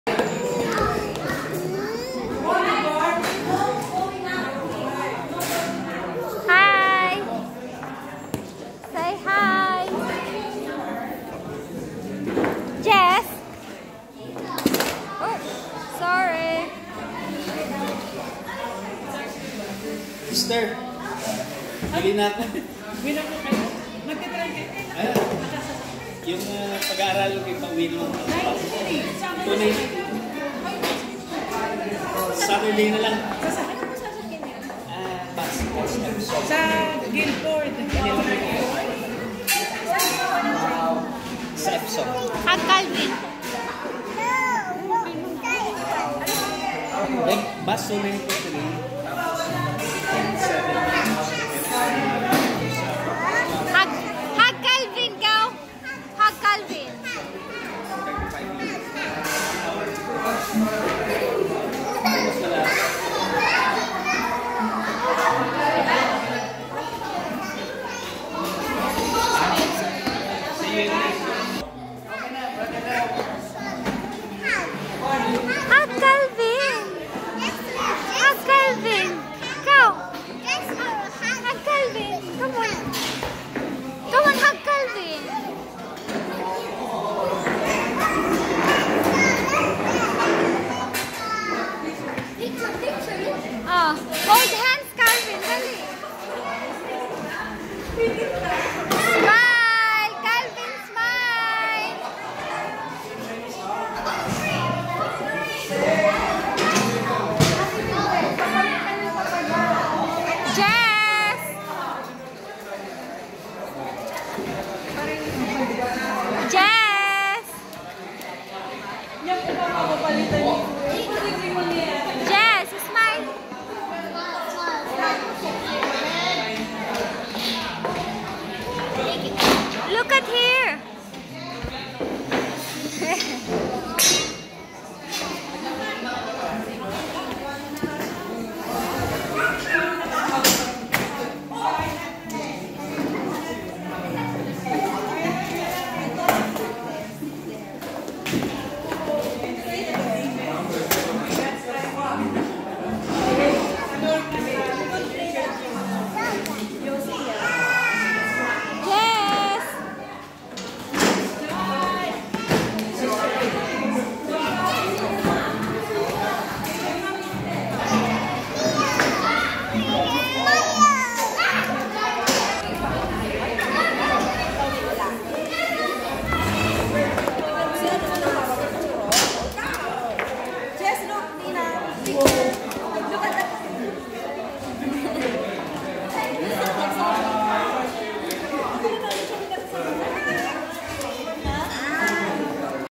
Children are blessings from GOD| Sunday Kids| After Service
Children are gift from the Lord; they are reward from Him. Psalm 127:3 NLT

My happy pill and stress reliever! My inaanak and other children playing around after Sunday service. Love to capture their moment. Glory to God!